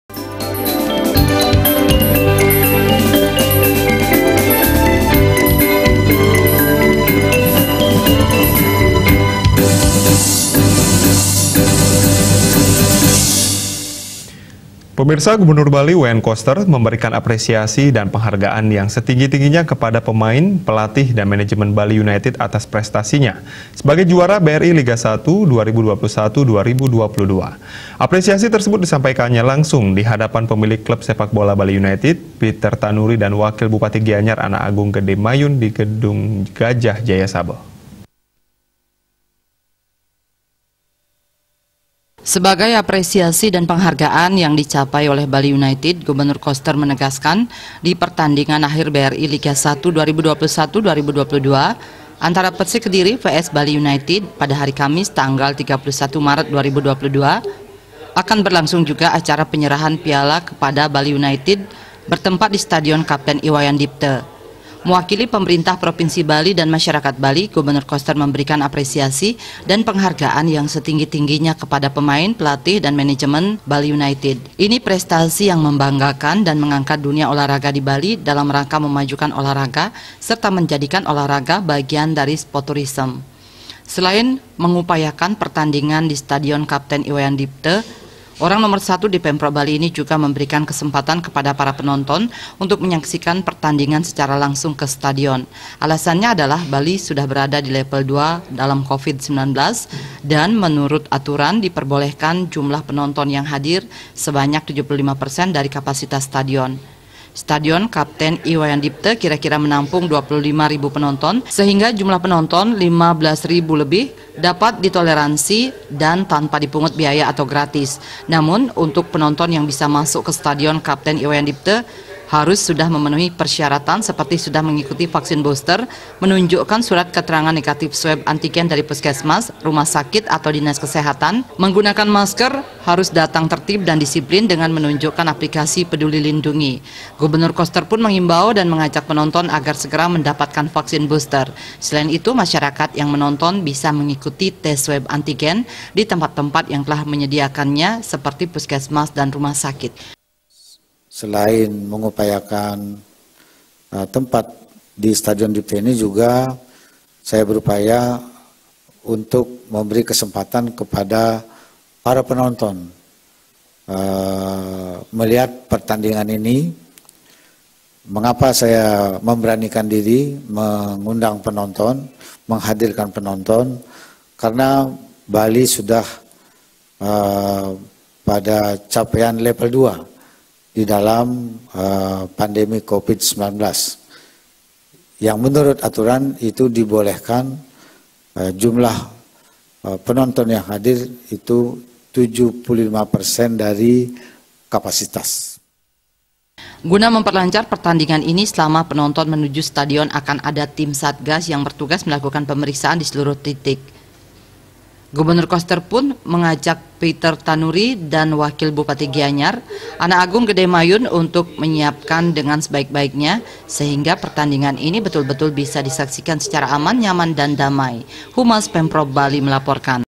GUBERNUR KOSTER APRESIASI BALI UNITED RAIH JUARA BRI LIGA I 2021–2022
GUBERNUR BALI, WAYAN KOSTER MEMBERIKAN APRESIASI, DAN PENGHARGAAN YANG SETINGGI-TINGGINYA KEPADA PEMAIN, PELATIH, DAN MANAJEMEN BALI UNITED ATAS PRESTASINYA SEBAGAI JUARA BRI LIGA SATU 2021–2022. APRESIASI TERSEBUT DISAMPAIKANNYA LANGSUNG DIHADAPAN PEMILIK KLUB SEPAKBOLA BALI UNITED, PIETER TANURI DAN WAKIL BUPATI GIANYAR, ANAK AGUNG GDE MAYUN DI GEDUNG GAJAH, JAYASABHA.